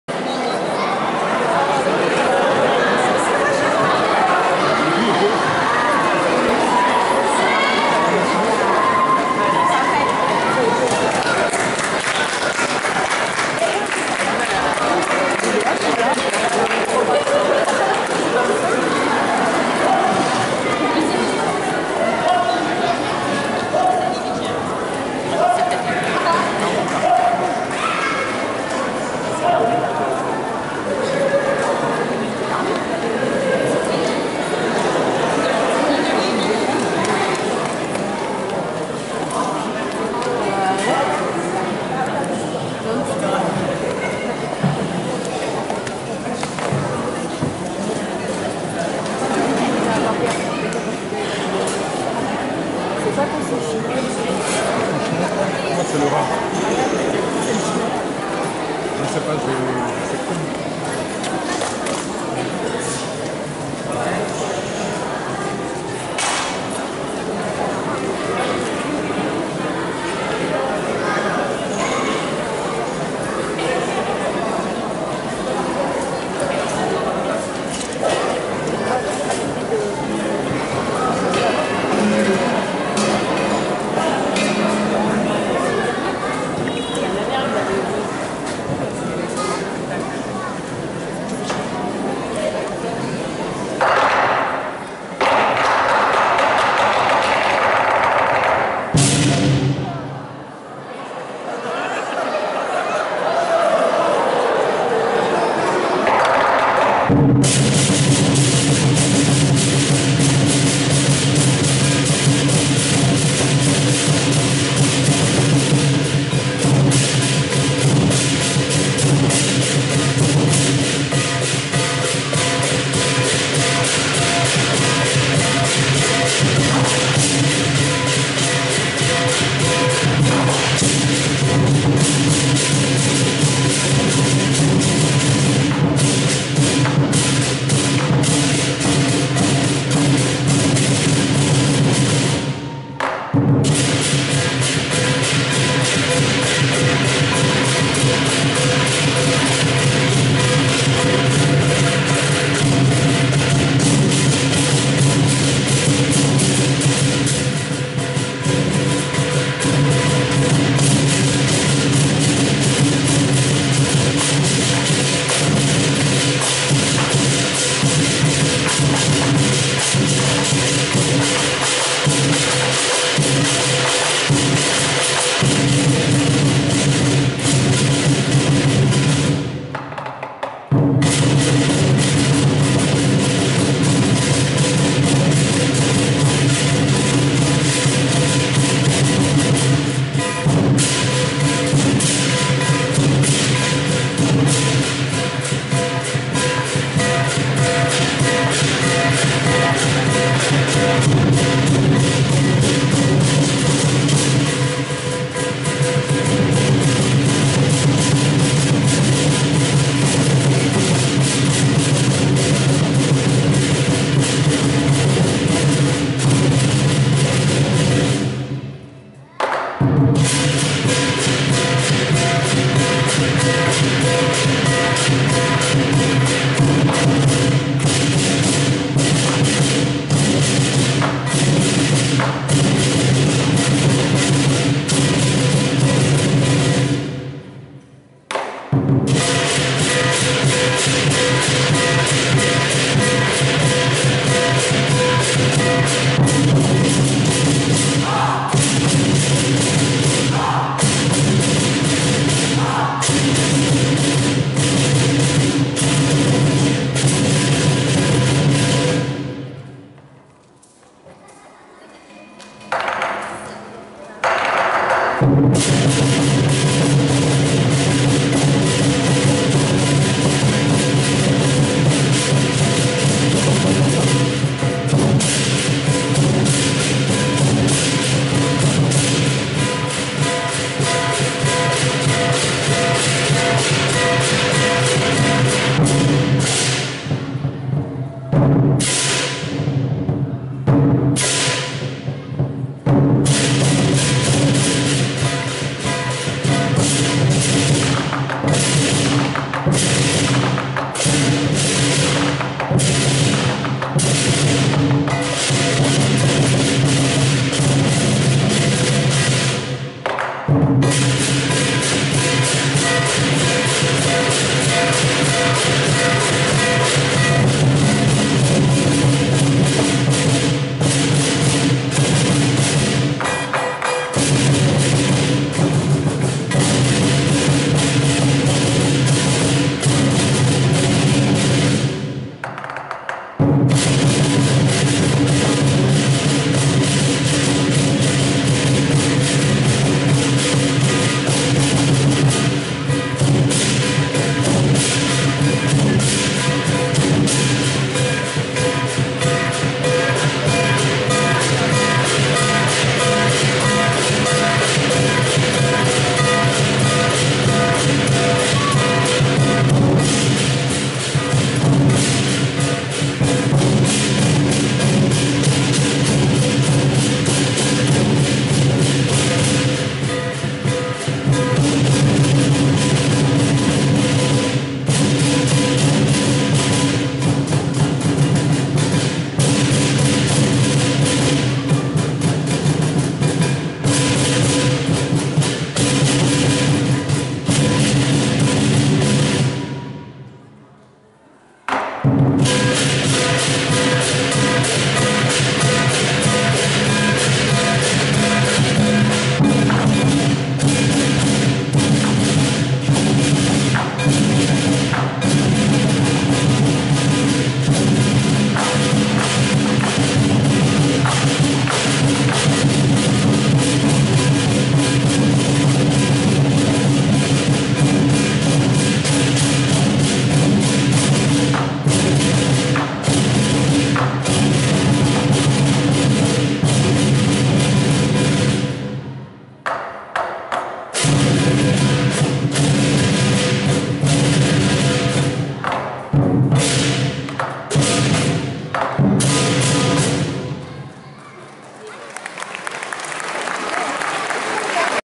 Nouvel an chinois 2016 au Gros Saule à Aulnay-sous-Bois !
Nouvel an chinois 2016 au Gros Saule à Aulnay-sous-Bois ! Cosec du Gros Saule avec l'ACSA. Le 20 février 2016.